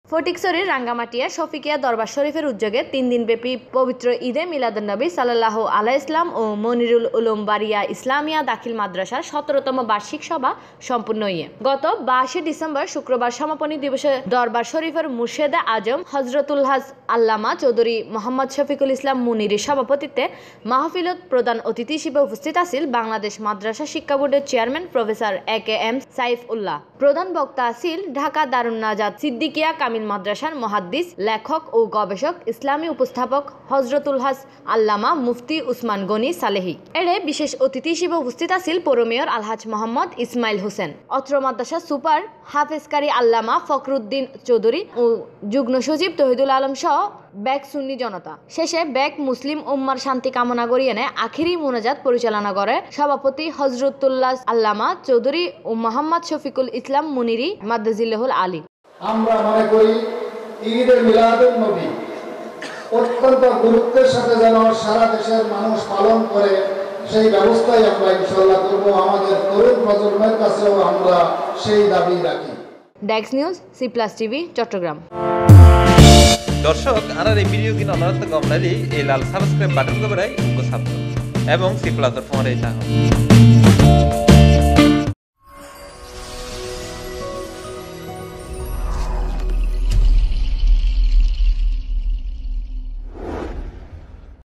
ফটিকছড়ির শফিকীয়া দরবার শরীফে ৩ দিনব্যাপী পবিত্র জশনে ঈদ-এ মিলাদুন্নবী (স:) সম্পন্ন
ফটিকছড়ির রাঙ্গামাটিয়া শফিকীয়া দরবার শরীফের উদ্যোগে ৩ দিন ব্যাপী পবিত্র ঈদ-এ মিলাদুন্নবী (স:) ও মুনিরুল উলুম বারীয়া ইসলামিয়া দাখিল মাদ্রাসার ১৭তম বার্ষিক সভা সম্পন্ন হয়েছে।

গত ২২ ডিসেম্বর শুক্রবার সমাপনি দিবসে দরবার শরীফের মুর্শেদে আজমক হযরতুলহাজ্ব আল্লামা চৌধুরী মো: শফিকুল ইসলাম মুনিরীর সভাপতিত্ত্বে মাহফিলে প্রধান অতিথি হিসেবে উপস্থিত ছিলেন বাংলাদেশ মাদ্রাসা শিক্ষা বোর্ডের চেয়ারম্যান প্রফেসর এ.কে.এম. ছায়েফ উল্যাহ।
প্রধান বক্তা ছিলেন ঢাকা দারুন্নাজাত ছিদ্দিকীয়া কামিল মাদ্রাসার মুহাদ্দিস, লেখক ও গবেষক এবং চ্যানেল আই ও মাই টিভি ইসলামী উপস্থাপক, হযরতুলহাজ্ব আল্লামা মুফতি ওসমান গণি ছালেহী।এতে বিশেষ অতিথি হিসেবে উপস্থিত ছিলেন পৌর মেয়র আলহাজ্ব মো: ইসমাইল হোসেন, উপজেলা আওয়ামীলীগের সাধারণ সম্পাদক নাজিম উদ্দিন মুহুরী, আওয়ামীলীগ নেতা শিল্পপতি সাদাত আনোয়ার সাদী, অত্র মাদ্রাসার সুপার হাফেজ ক্বারী আল্লামা ফখরউদ্দীন চৌধুরী, বারীয়া শফিকুল মুনীর যুব কমিটি বাংলাদেশের চেয়ারম্যান শাহ্জাদা আলহাজ্ব সালাহউদ্দীন চৌধুরী, মহা সচিব মুহাম্মদ নজরুল ইসলাম ও যুগ্ন সচিব তৌহিদুল আলমসহ সকল সুন্নী-জনতা।

শেষে সকল মুসলিম উম্মার শান্তি কামনা করে আখেরি মুনাজাত পরিচালনা করেন সভাপতি হযরতুলহাজ্ব আল্লামা চৌধুরী মুহাম্মদ শফিকুল ইসলাম মুনিরী (ম.জি.আ.)।